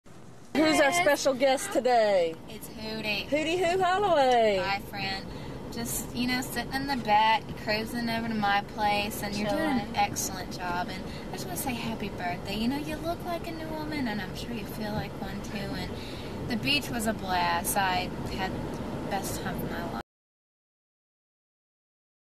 Natalee Holloway Home Video
rare home video of Natalee Holloway aka 'Hootie'. its one of the few public videos that show Natalee's voice and her southern accent.